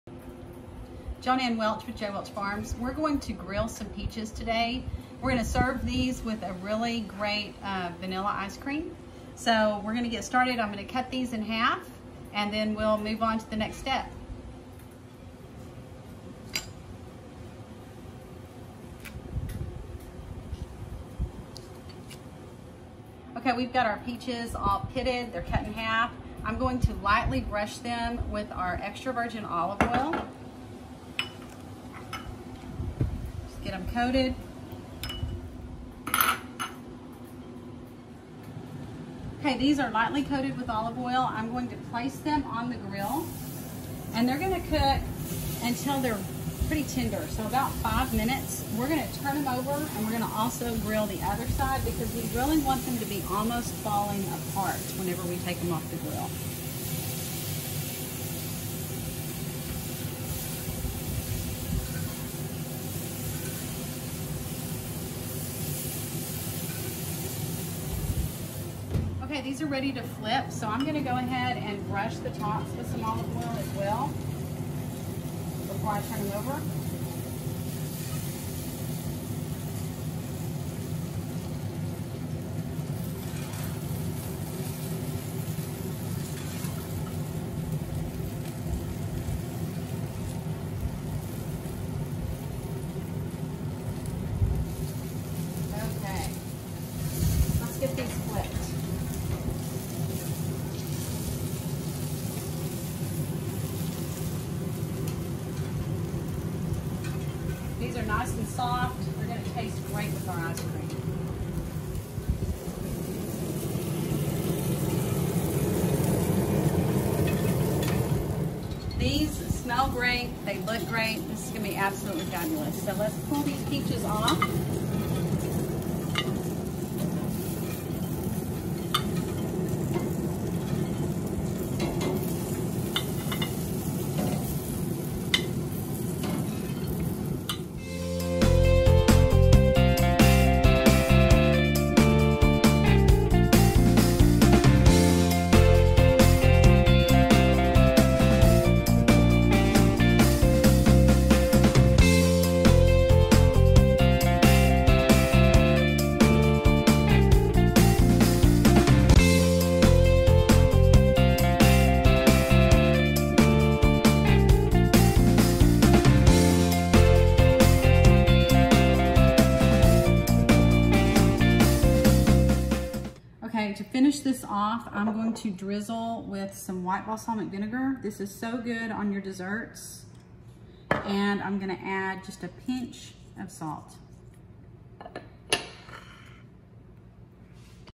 Grilled Peaches | Great Summer Grilling Dish | EVOO
Learn how to make a Grilled Peaches Recipe! for the ingredients! Plus, more info and more recipes. I hope you enjoy this Grilled Peaches recipe from our farm to your table!